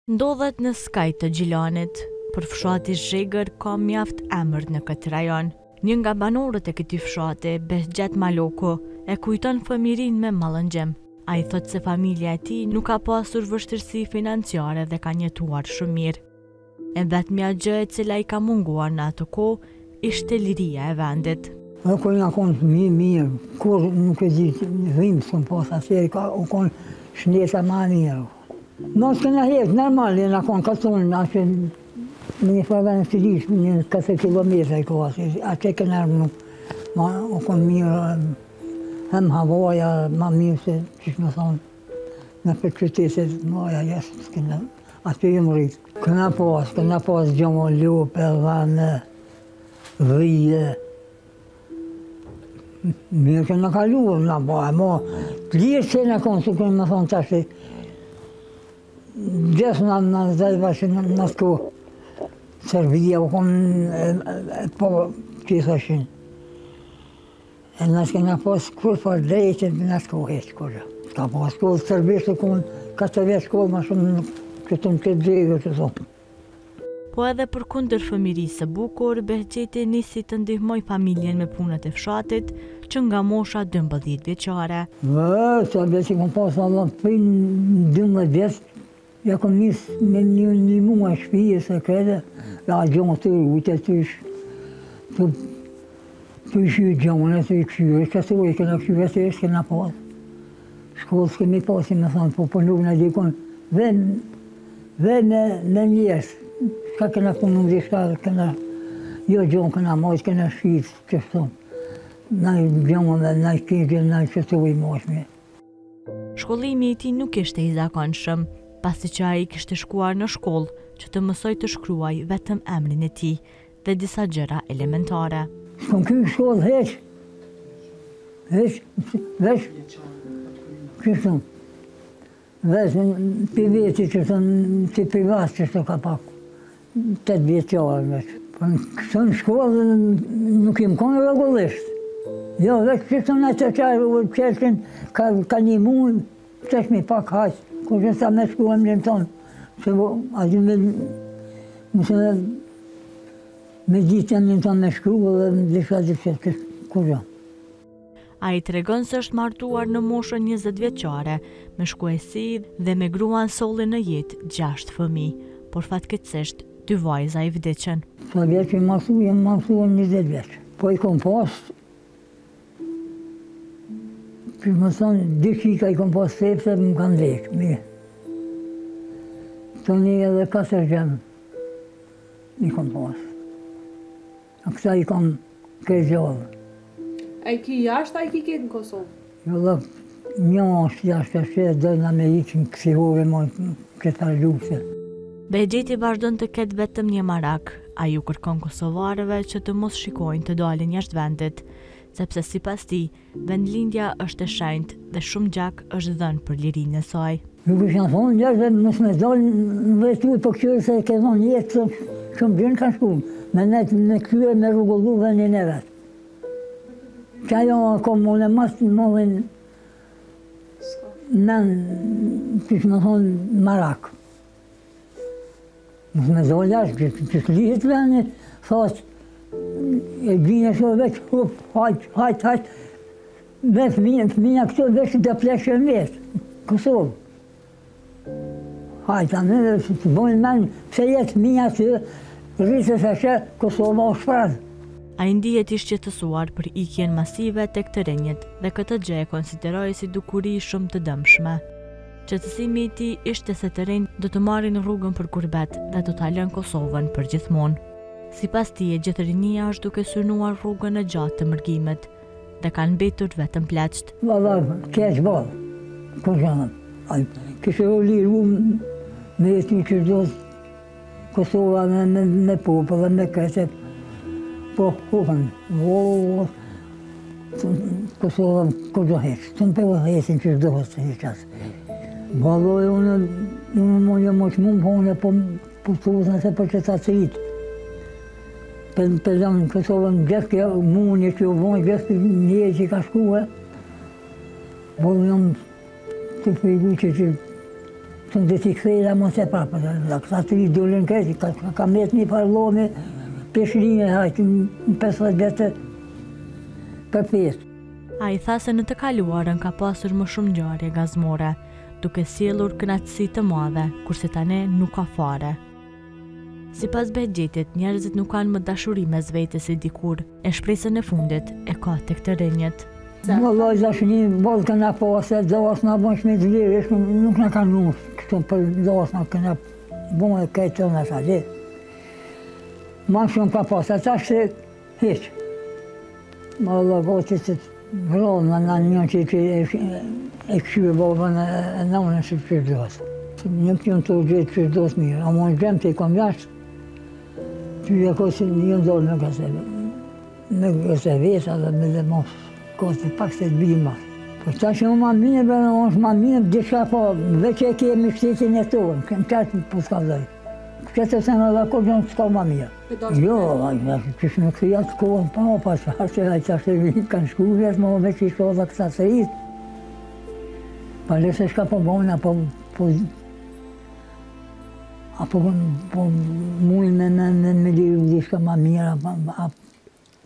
Nuk kemi pasur kurrfarë të drejte në kohë të Serbisë" - Tregime nga katuni
"Nuk kemi pasur vështirësi financiare, por e vetmja gjë që na ka munguar ka qenë liria e vendit, nuk kemi pasur kurrfarë të drejte në kohë të Serbisë" - Tregime nga katuni